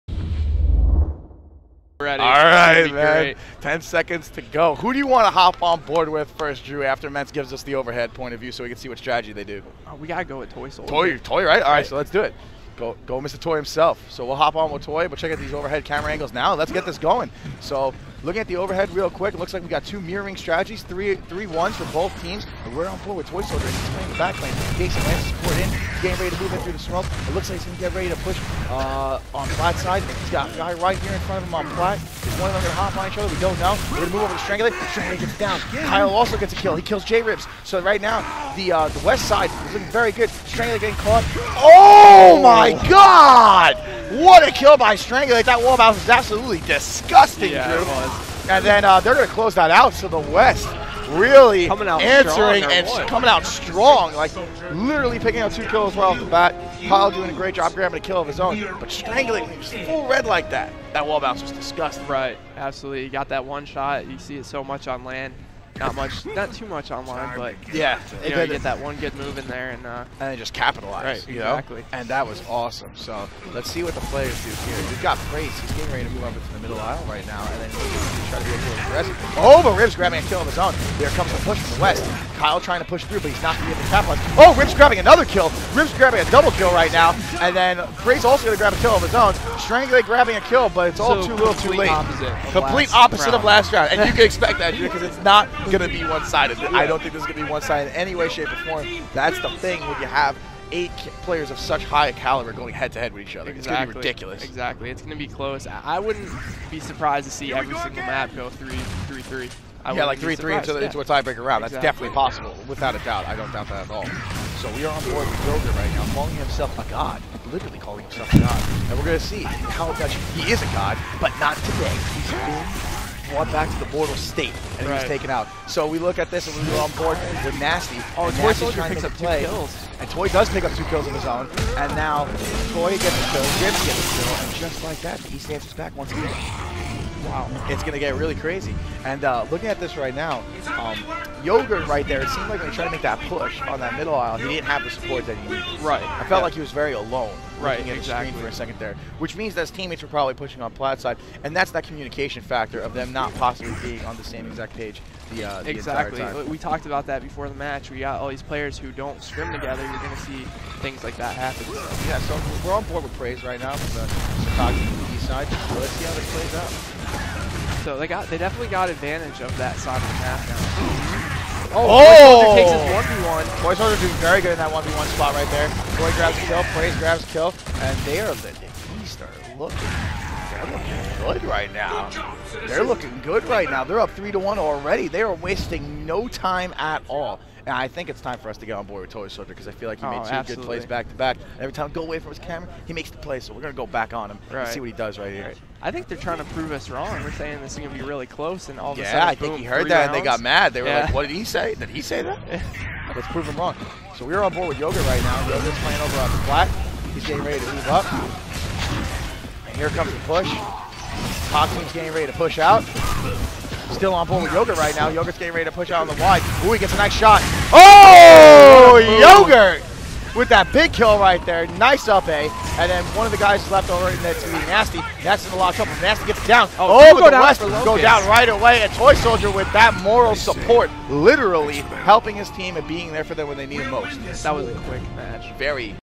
Hype SJ: All-Star Match East vs West (Checkout)
Hypefestation Summer Jam held in Philadelphia, PA August 29-31 -- Watch live